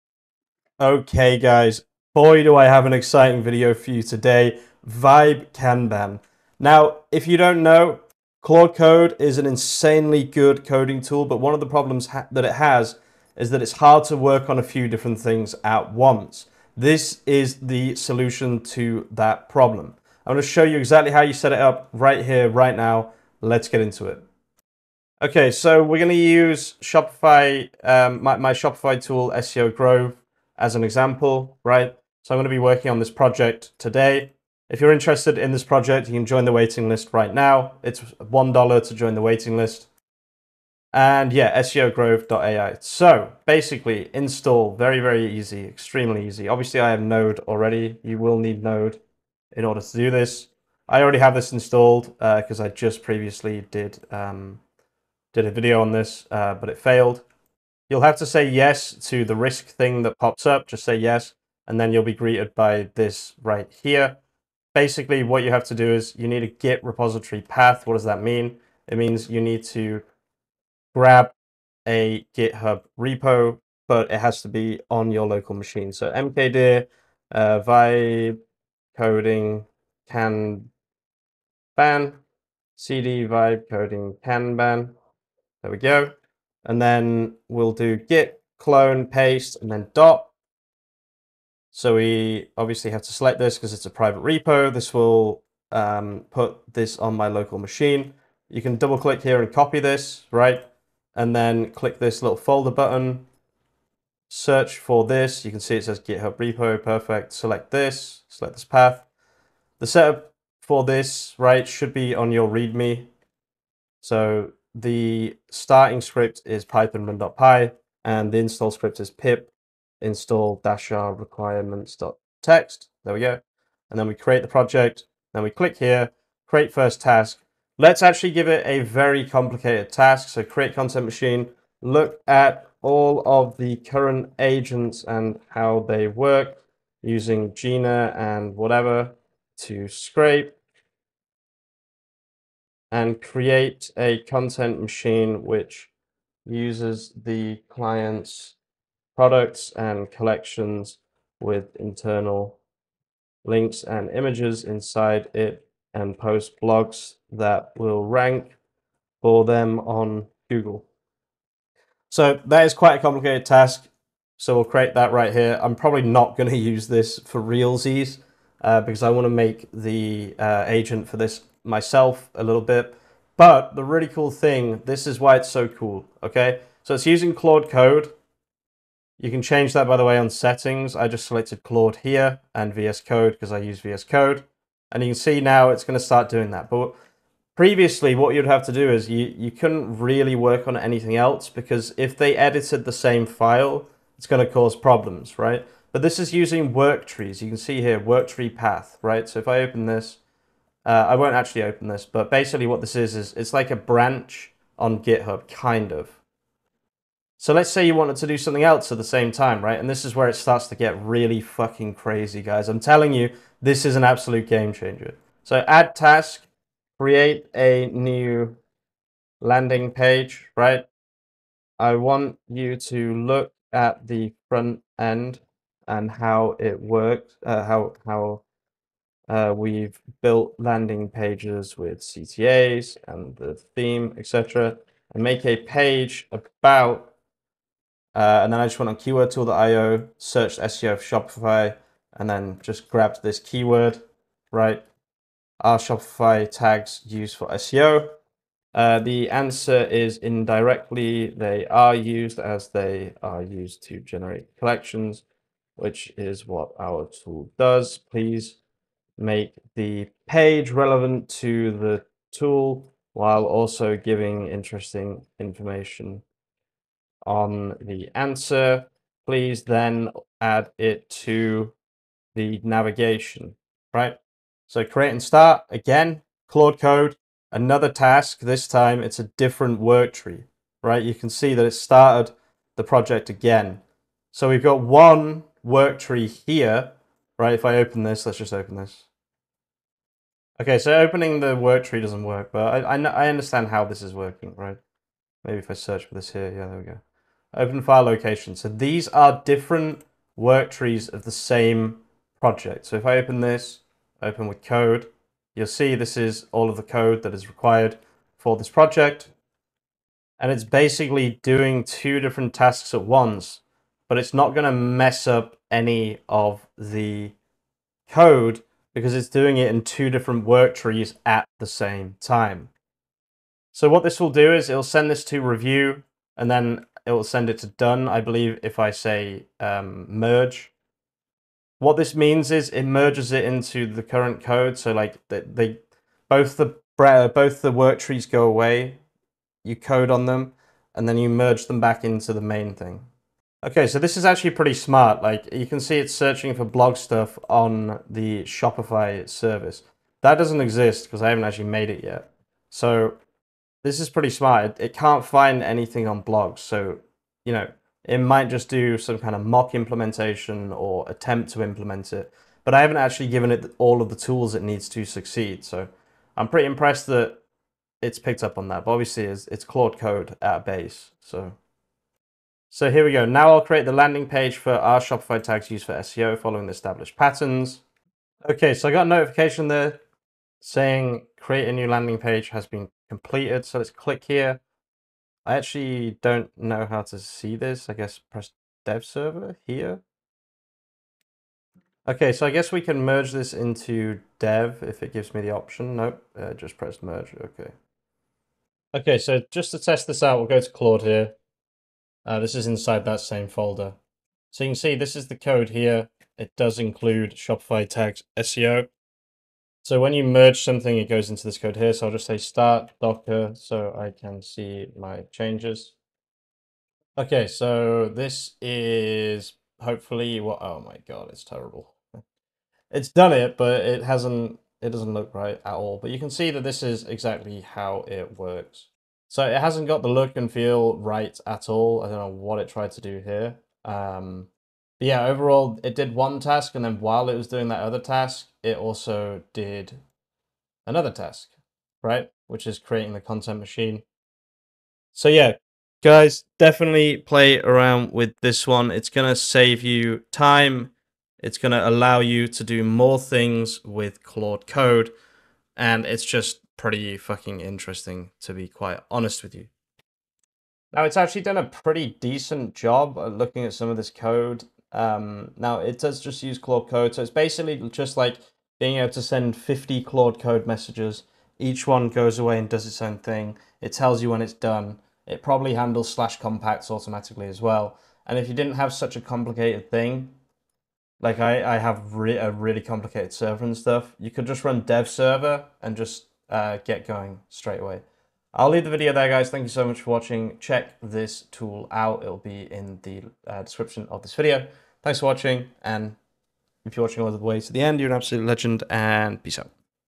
Vibe-Kanban: Claude Code Just Got An INSANE Upgrade (SUPERCHARGED)
🚀 GAME CHANGER: The Tool That Fixes Claude Code's Biggest Limitation!
Claude Code is incredible, but working on multiple tasks at once has always been impossible... until NOW! I'm showing you Vibe Coding Kanban - the revolutionary tool that lets you run unlimited Claude Code tasks simultaneously using Git work trees. This changes everything!

🔥 What Vibe Coding Kanban Solves:
✅ Run multiple Claude Code tasks simultaneously without conflicts
✅ Kanban board interface for managing AI coding projects
✅ Git work trees for isolated development branches
✅ Automatic merging when tasks complete
✅ No more waiting for one task to finish before starting another
💡 Live Demo Features:

SEO Grove Shopify tool development
Complex content machine creation task
Simultaneous landing page generation
Real-time work tree management
Automatic code merging and deployment

🛠️ Complete Setup Process:

Node.js requirement and installation
Local Git repository configuration
Project setup with install/start scripts
Claude Code integration settings
VS Code workspace management

📊 Real Example Tasks:

Content Machine: Complex multi-agent system using Gina scraping, client products, collections, internal links, and blog posting for Google rankings
Landing Page: SEO-focused page about "Shopify tags for SEO" with proper navigation integration

⚡ Technical Benefits:

Git work trees prevent code conflicts
Isolated development environments
Automatic compact handling
Seamless branch management
Production-ready merging process

🎯 Perfect For: Claude Code power users, developers managing complex projects, AI coding enthusiasts, anyone frustrated by single-task limitations
⚠️ Requirements:

Local Git repository setup
Node.js installation
Basic understanding of Claude Code workflows

💰 Business Impact: Build multiple features simultaneously, faster development cycles, more efficient AI-assisted coding

SEO-Optimized Chapters:
0:00 - 1:00 - Vibe Coding Kanban Introduction & Claude Code Limitation Solution
1:00 - 2:30 - Installation Setup & Git Repository Configuration Process
2:30 - 4:30 - Complex Content Machine Task Creation & Work Tree Demo
4:30 - 6:30 - Simultaneous Landing Page Task & Multi-Project Management
6:30 - 8:18 - Live Results Testing & Code Merging Demonstration